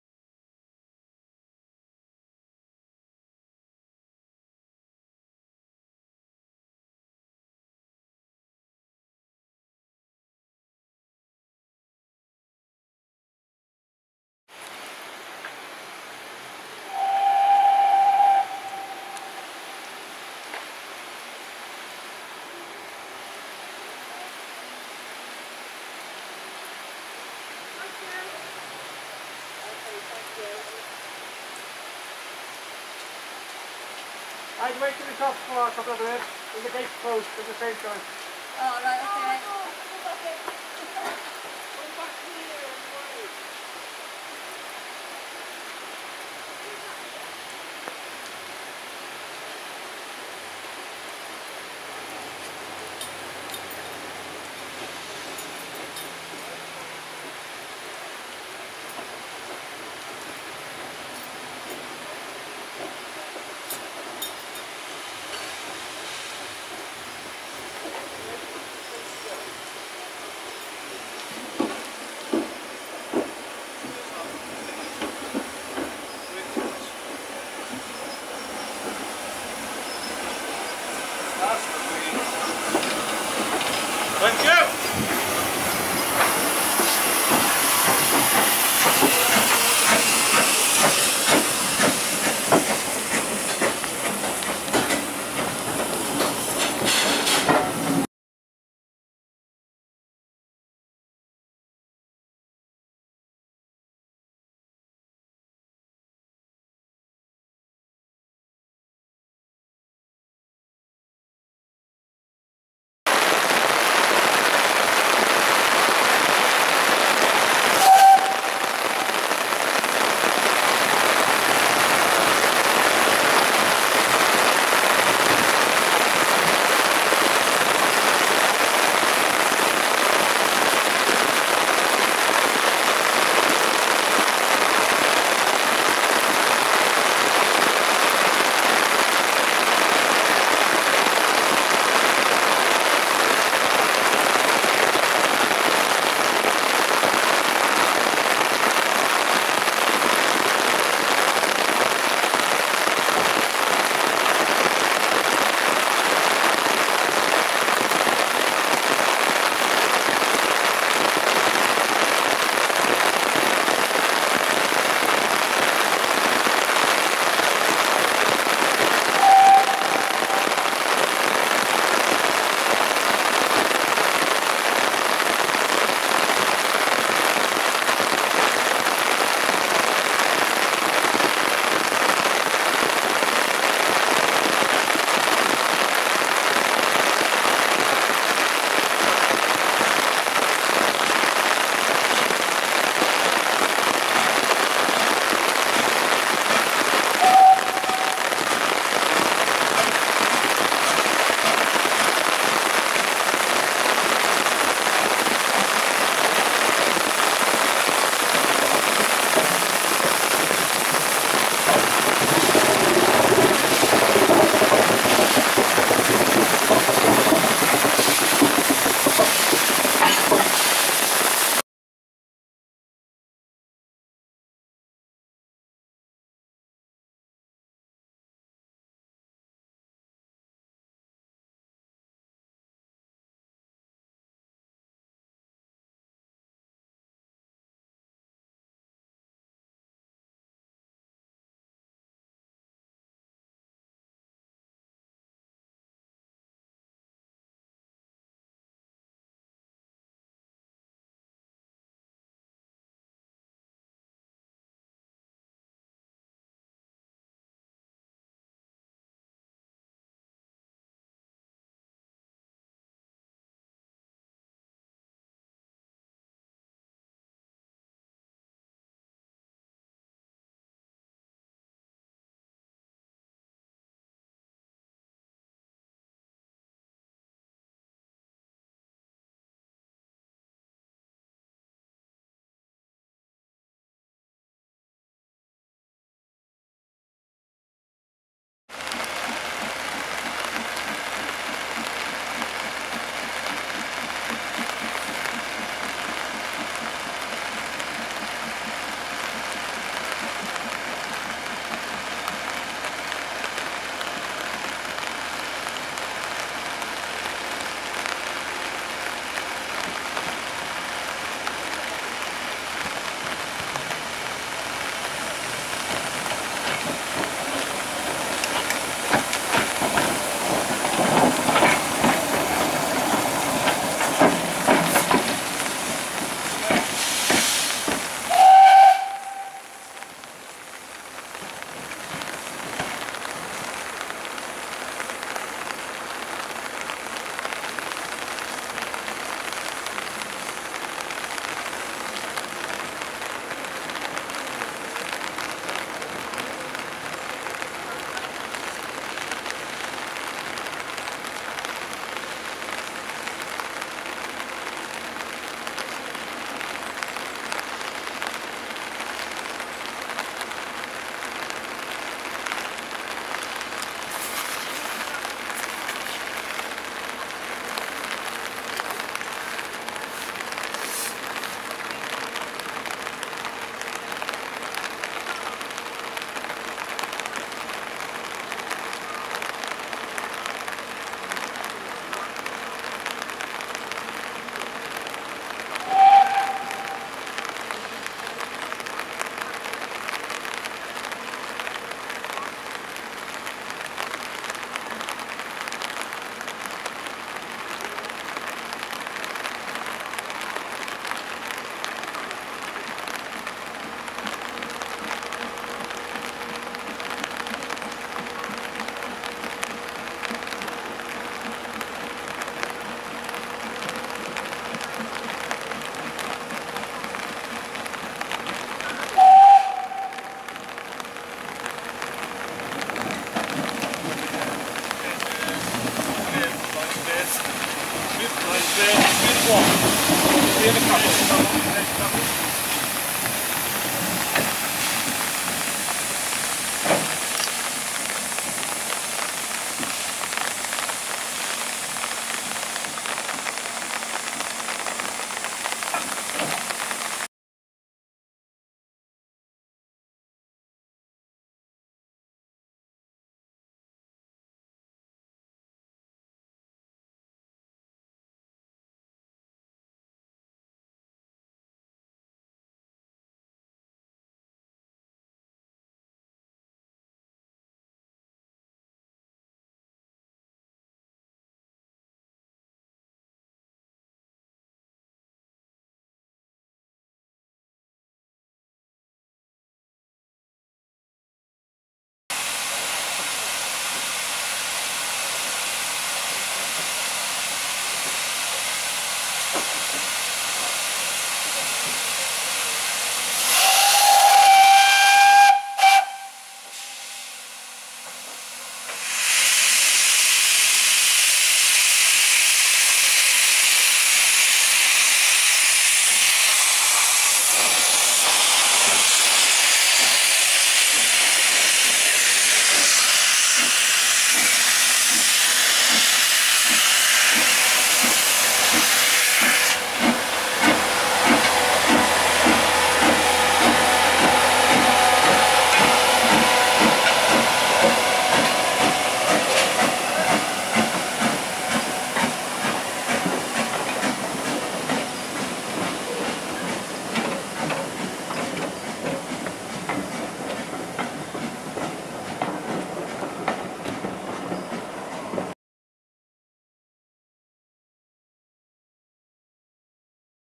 A Wet Gwili
A damp day on a great railway.